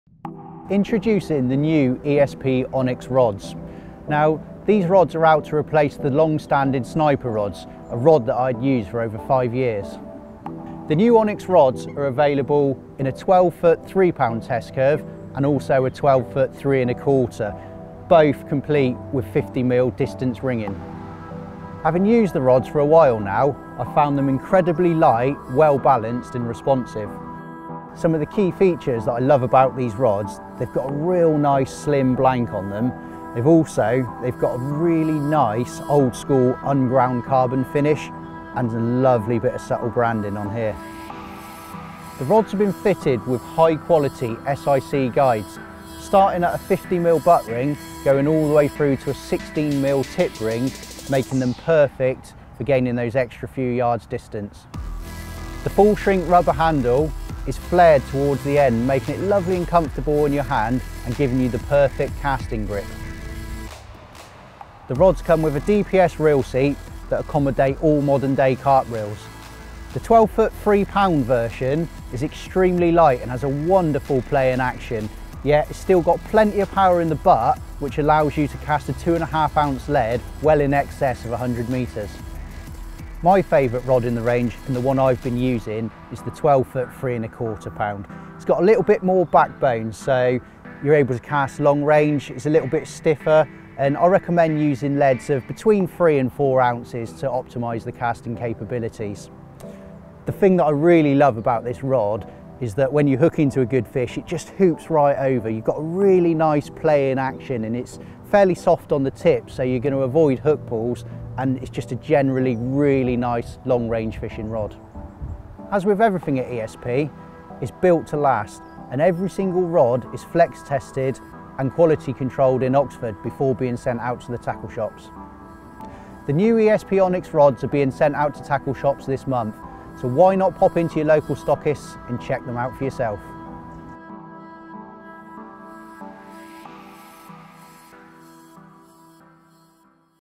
NEW ESP Onyx Rods
The new ESP Onyx rods are available in two popular test curves, 12’ 3.0lb and 12’ 3.25lb, both with 50mm distance ringing. Light, well balanced and responsive the slim high modulus blanks feature a traditional unground finish which gives them a nice ‘old school’ look.

The high quality, lightweight SiC guides are arranged in a long range ringing pattern comprising a 50mm butt and single leg intermediates down to a 16mm tip. The full length shrink rubber handle features an ergonomic and aesthetically pleasing flared casting grip and a gunsmoke DPS reel fitting with anodized butt cap and collars. Subtle decals and a small ESP logo on the line clip whipping complete a nice understated look.